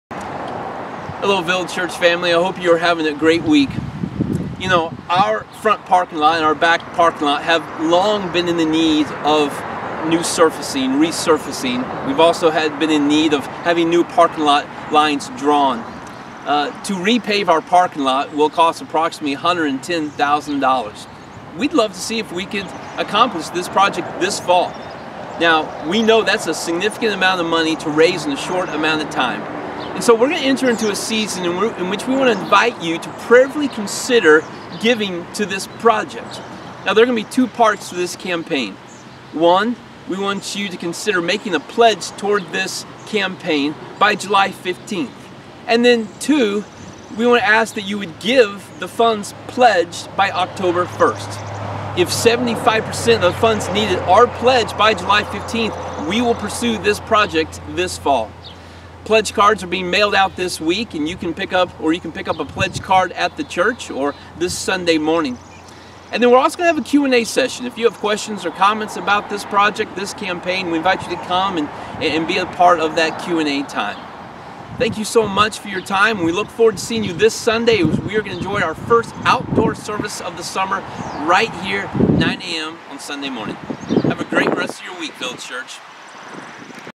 Pave The Parking Lot Campaign
Jeff Horch invites Village Church family to prayerfully consider giving toward the parking lot campaign to repave the parking lot.